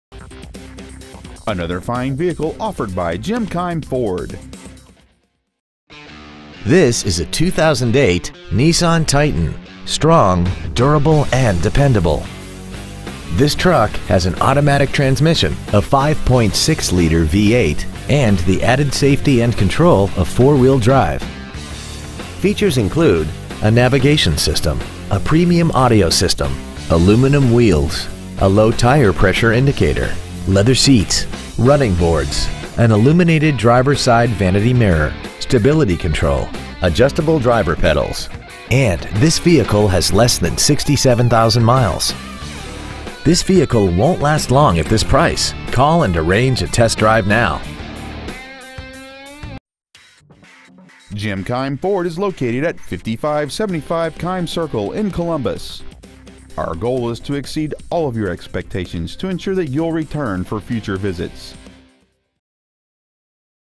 2008 Titan Columbus OH
We are proud to present this 2008       Titan .

 Year : 2008
 Make :
 Model : Titan
 Engine : 5.6L 8-Cylinder
 Trans . : automatic
 Exterior : Galaxy Black Metallic
 Miles : 66,953
 Interior : Charcoal

 Pre-Owned 2008       Titan Columbus OH